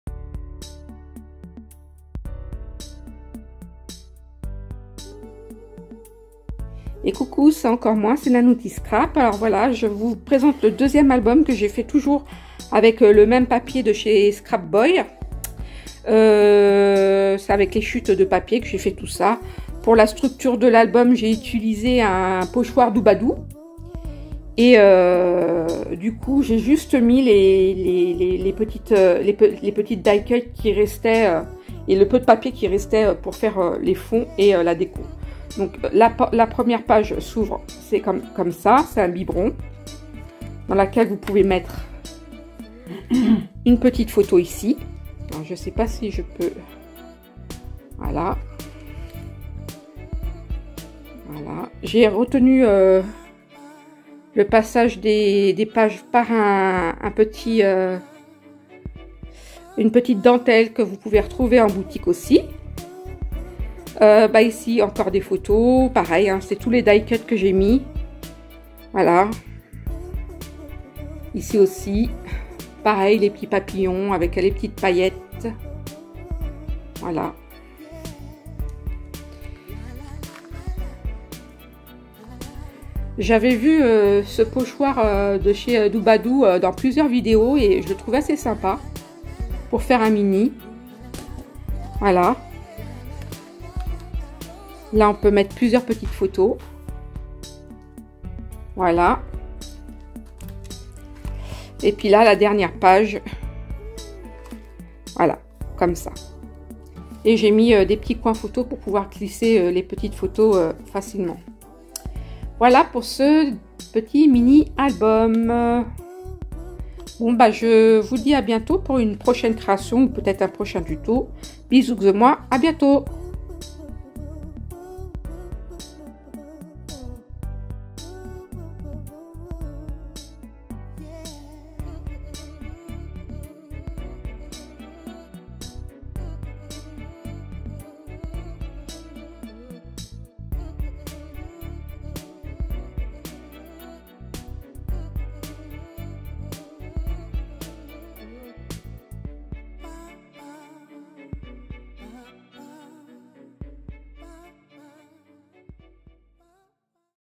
Mini album Scrap boy pour la boutique Scrap Merveilleux
Hello voici la 2ème présentation du mini album Scrap boy. Bizoox de moi.